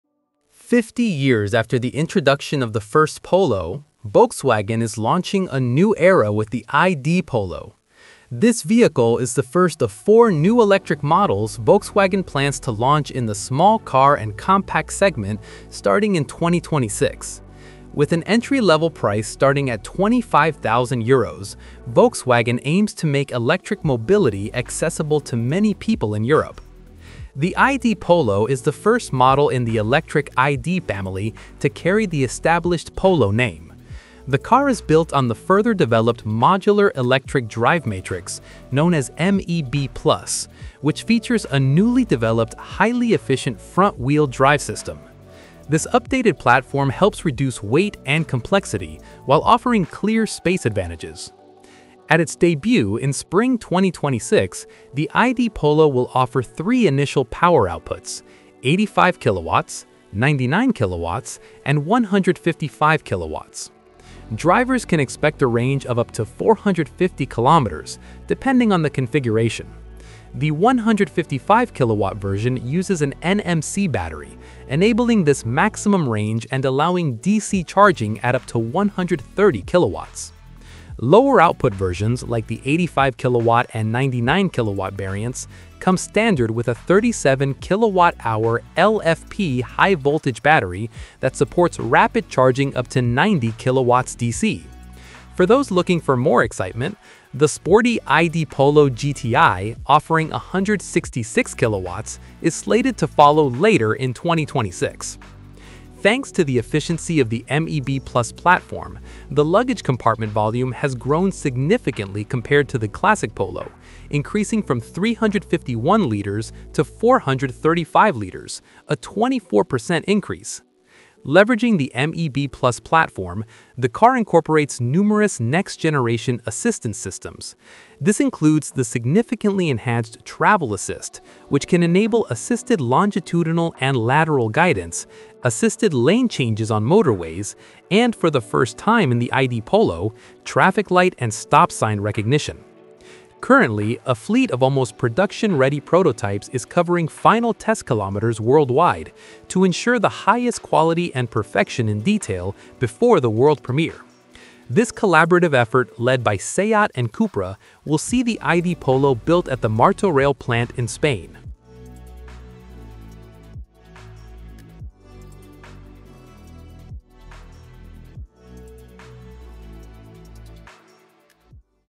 NEW Volkswagen ID.Polo 2026 (Proto) - Price & Specs Revealed!
New entry-level mobility – The ID. Polo is the first of four new Volkswagen electric models in the small car and compact segment, which will be launched from 2026 onwards.Technical Specifications Summary for the ID. Polo (2026)

• Initial Power Outputs (Three options): 85 kW (116 PS), 99 kW (135 PS), and 155 kW (211 PS).
• Sporty Variant: ID. Polo GTI (following later in 2026) offering 166 kW (226 PS).
• Maximum Range: Up to 450 km (depending on configuration).

Battery and Charging
• Lower Output Battery (85 kW / 99 kW): 37 kWh (net) LFP (lithium iron phosphate) high-voltage battery.
    ◦ DC Rapid Charging: Up to 90 kW.
• Higher Output Battery (155 kW / 166 kW): 52 kWh (net) NMC (nickel manganese cobalt) unified cell.
    ◦ DC Charging: Up to 130 kW.

Dimensions and Capacity
• Length: 4,053 mm.
• Width: 1,816 mm.
• Height: 1,530 mm.
• Wheelbase: 2,600 mm.

• Entry-Level Price: Starting at 25,000 euros.

Source : Volkswagen